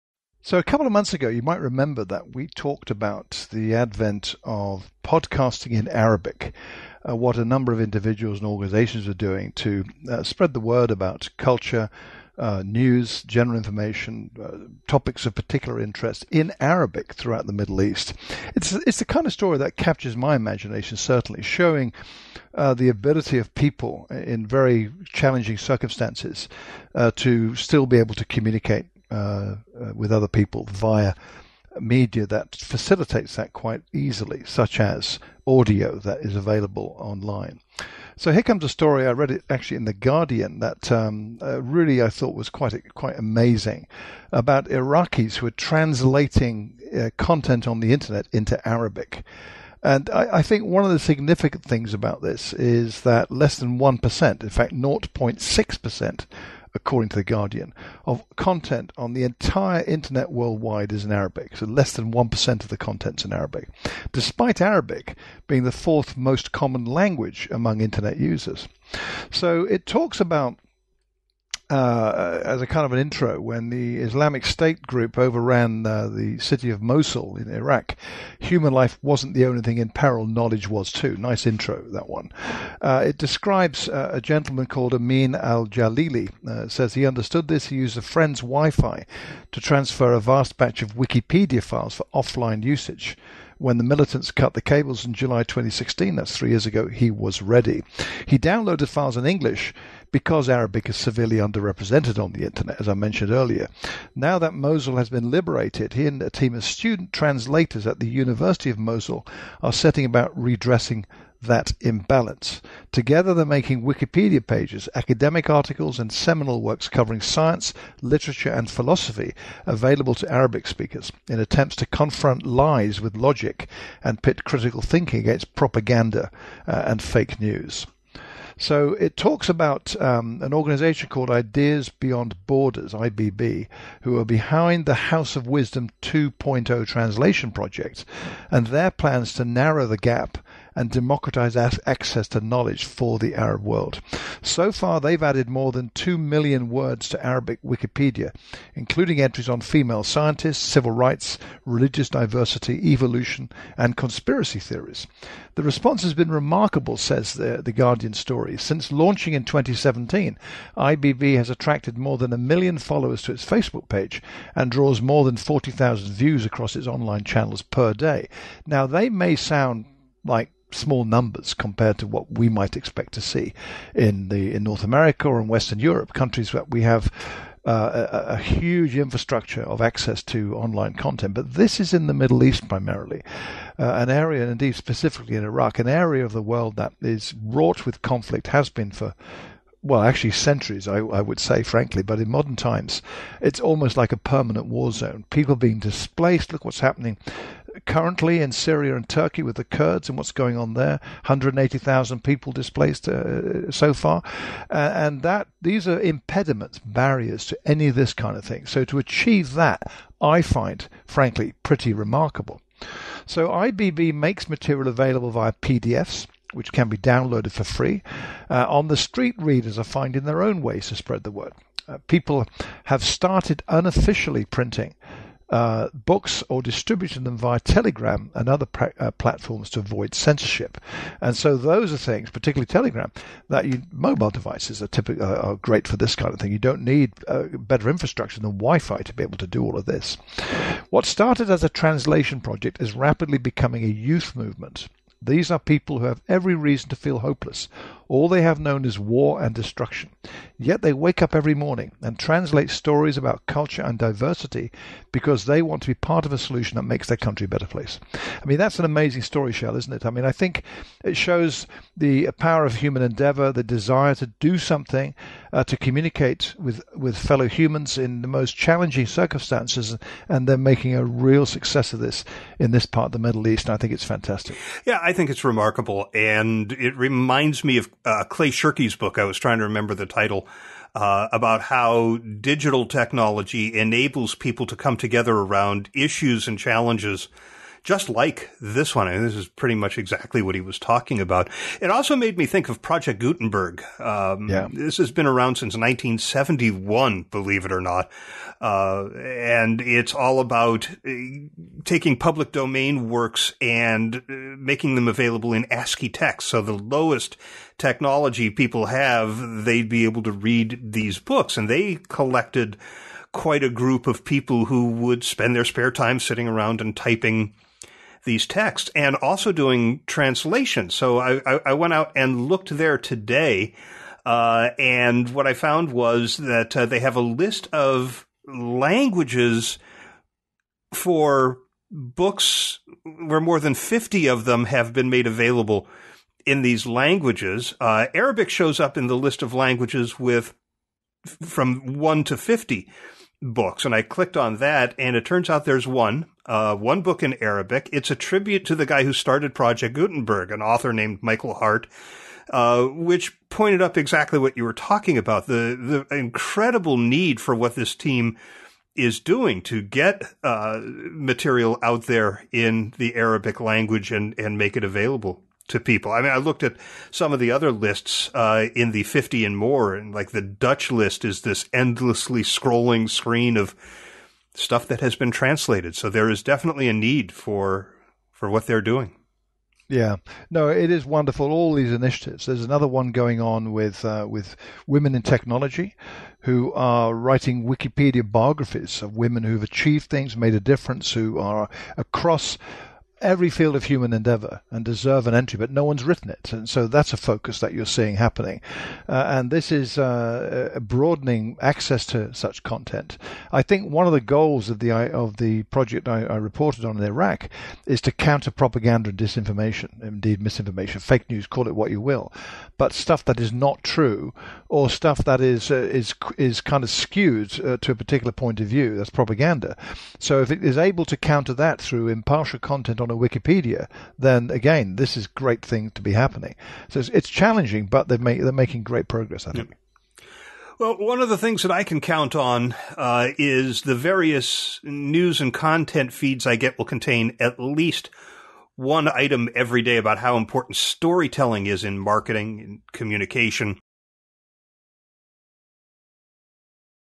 Ideas Beyond Borders - Translating the Internet into Arabic
"Just 0.6% of Internet content is available in Arabic despite it being the fourth most common language among internet users, according to a report by the Guardian.

What some people like Ameen al-Jaleeli in the Iraqi city of Mosul, and organisations such as Ideas Beyond Borders (IBB), are doing about this to “democratise access to knowledge for the Arab world” forms the basis for a great discussion Shel and I enjoyed in The Hobson and Holtz Report for October, aka FIR 188.

So far the organisation has added more than 2.1 million words to Arabic Wikipedia, including entries on female scientists, civil rights, religious diversity, evolution and conspiracy theories."

The response has been remarkable. Since launching in 2017, IBB has attracted more than 1 million followers to its Facebook page, and draws more than 40,000 views across its online channels a day.